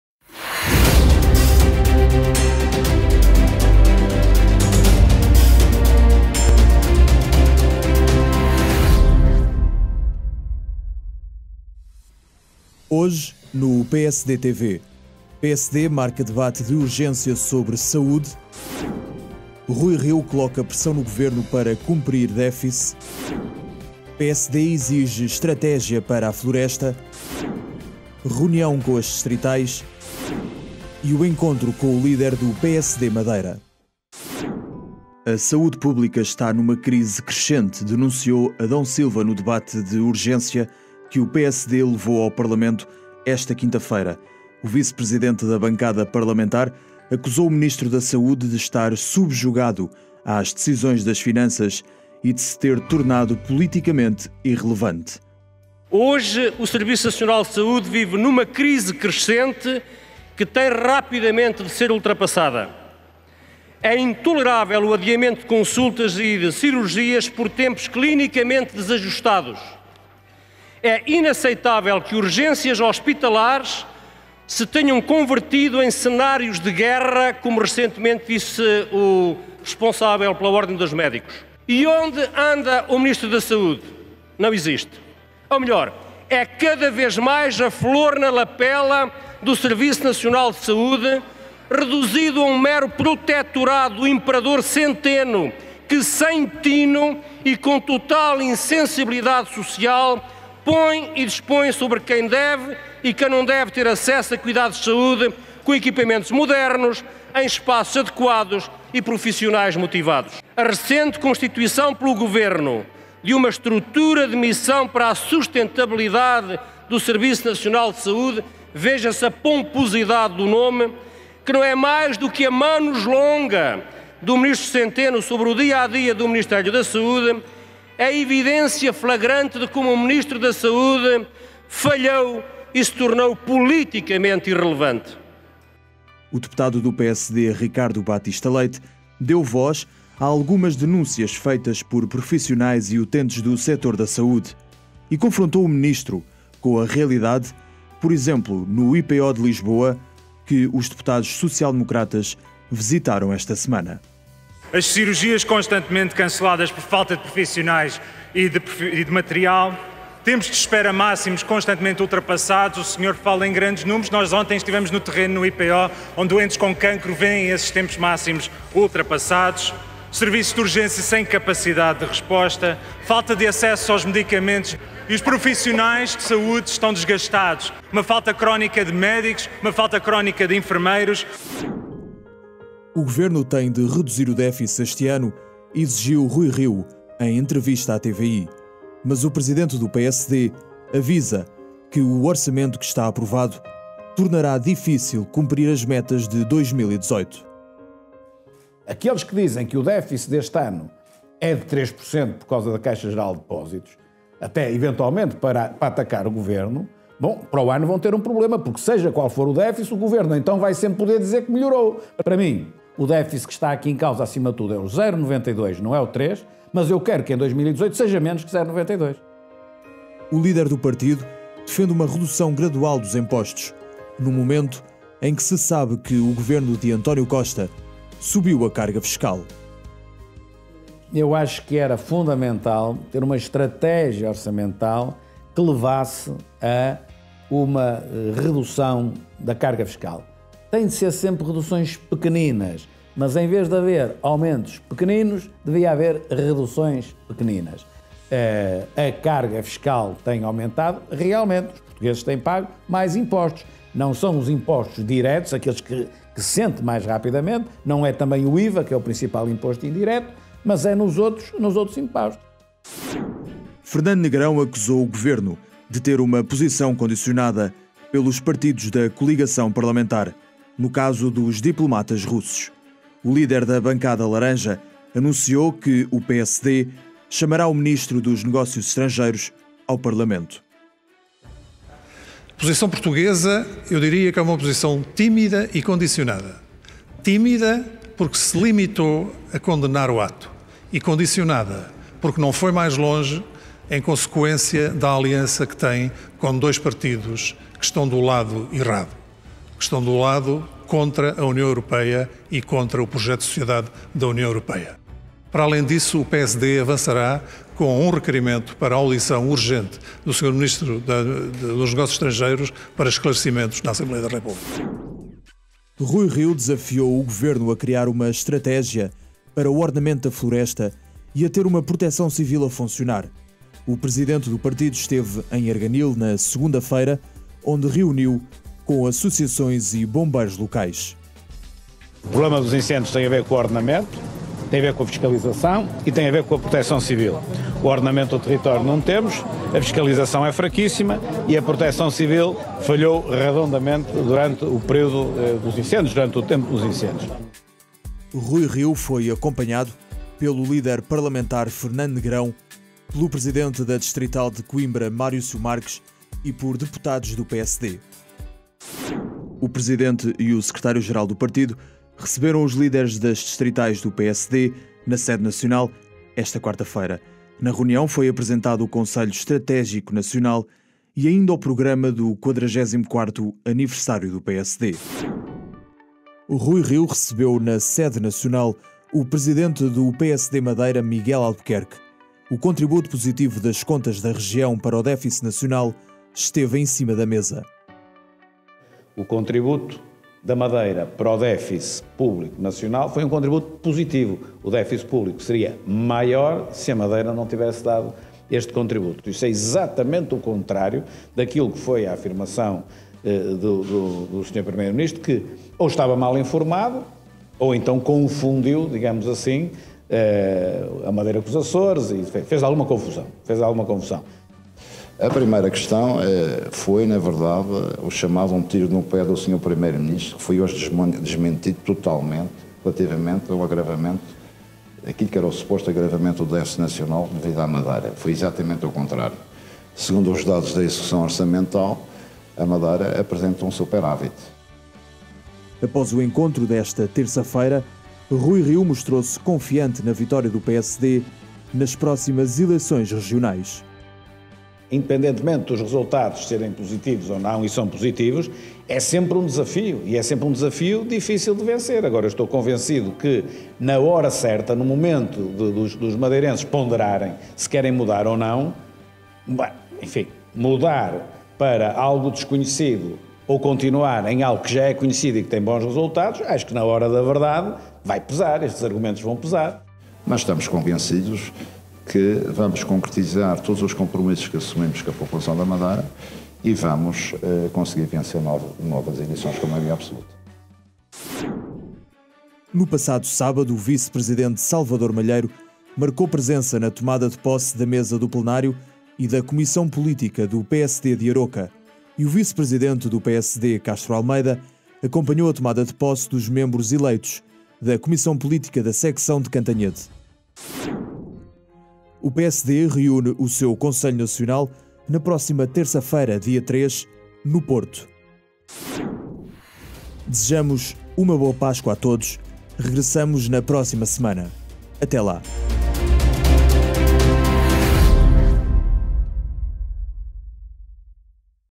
PSDTV 267
Hoje, no PSD@TV:

PSD marca debate de urgência sobre saúde;
Rui Rio defende coloca pressão no Governo para cumprir défice;
PSD exige estratégia para a floresta;
Reunião com as distritais;
E encontro com o líder do PSD Madeira.